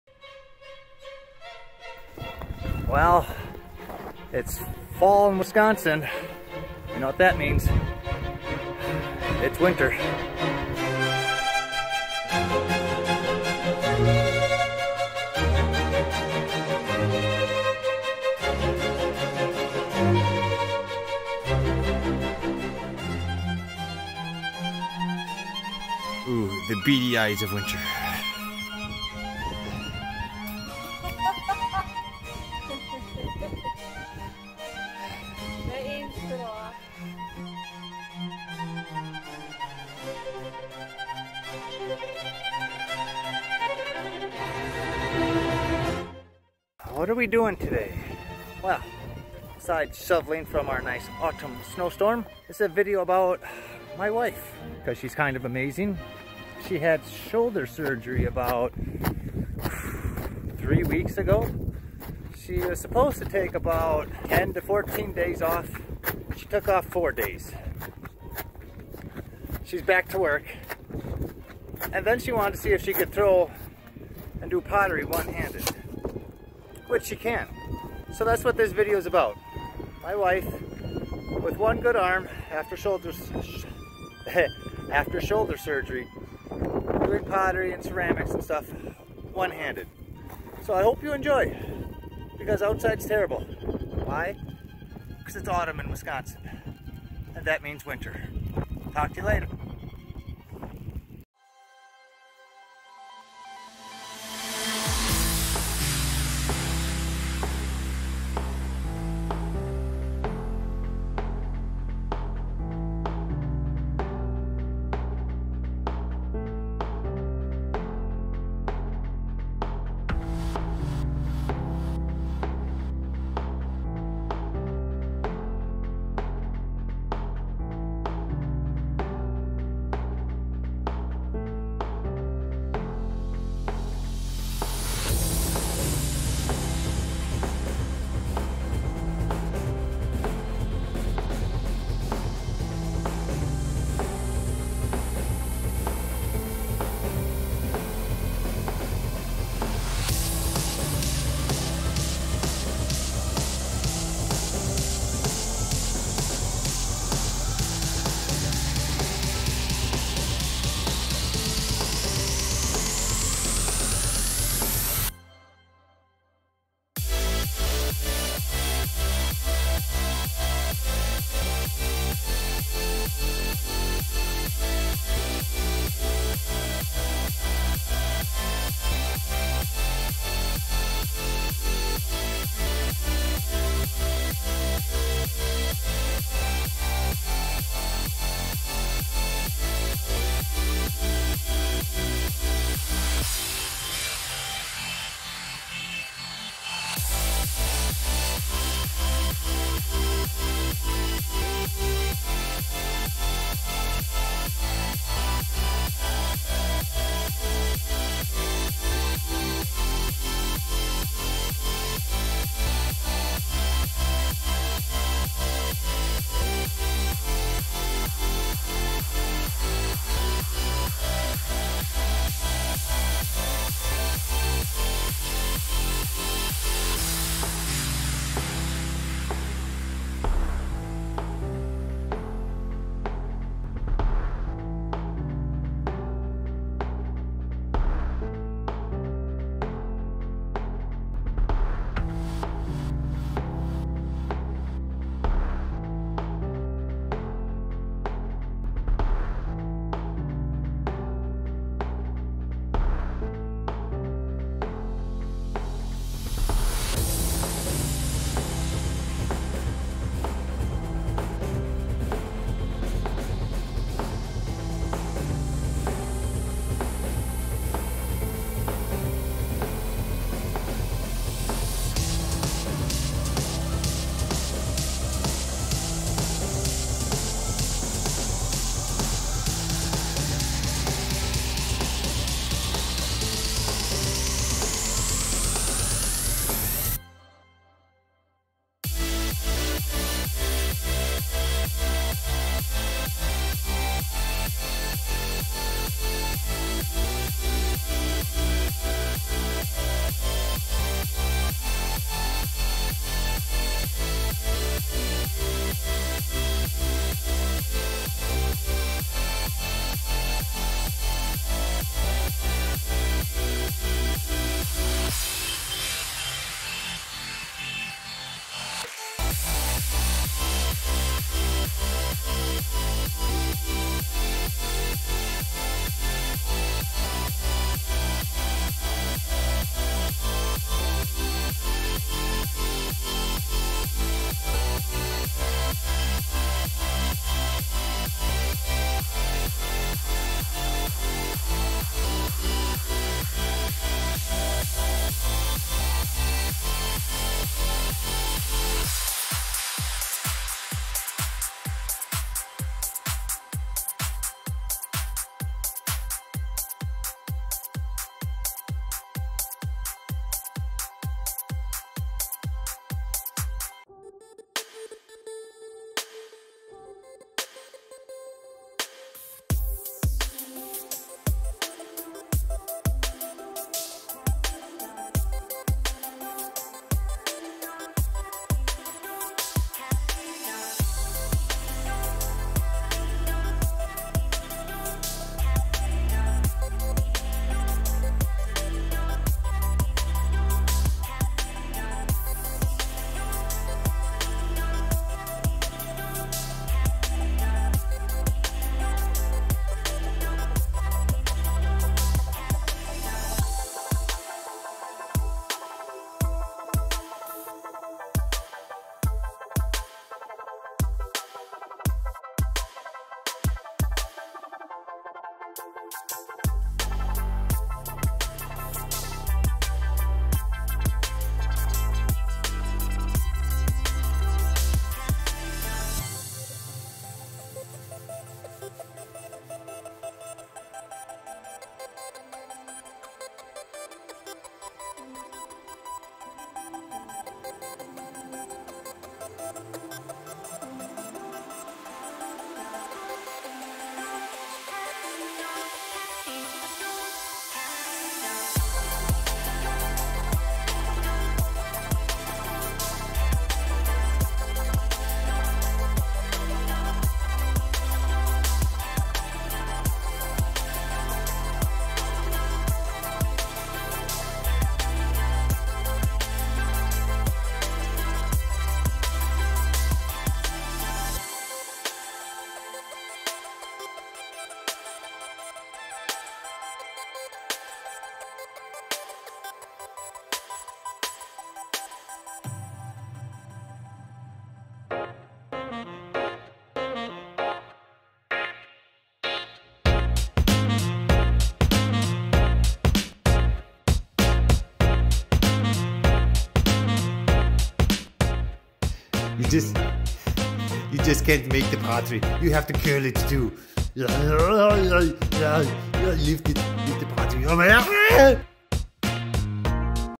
Beau and Kara Studios Episode 20 Throwing one handed
Kara doing pottery/ceramics with one arm after shoulder surgery.

Visit out website for more information on what we make and what it actually looks like.

Send Mail to:

Beau and Kara Lasiewicz

N290 County Road C